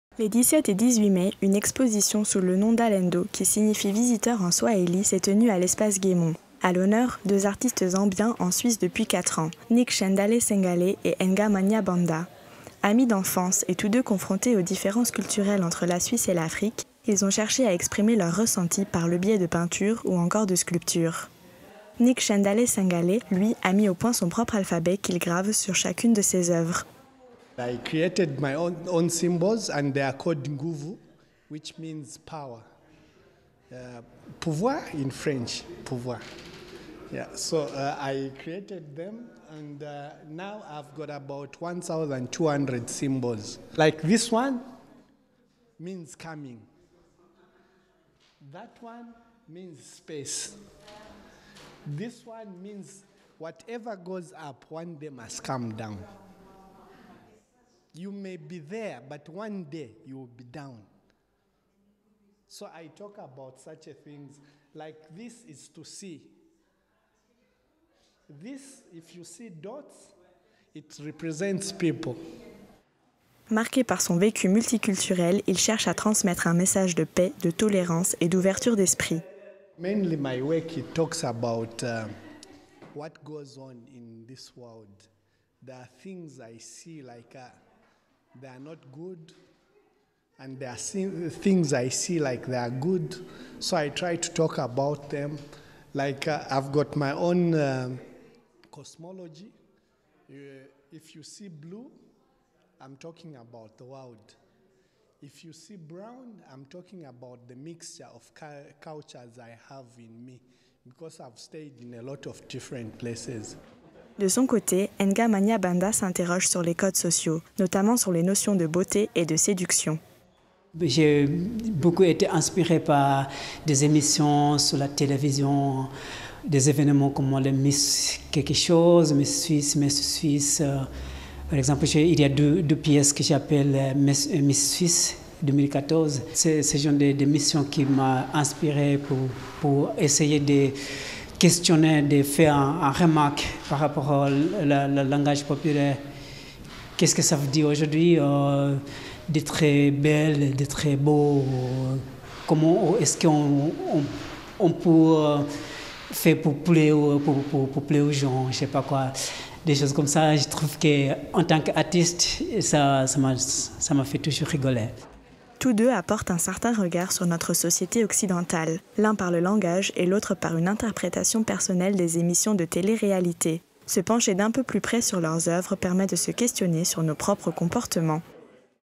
LANCY TV - Sujet - Alendo: deux artistes Zambiens s'interrogent sur les codes sociaux occidentaux.
Les 17 et 18 Mai, une exposition sous le nom de « Alendo », qui signifie visiteur en Swahili, s'est tenue à l'espace Gaimont.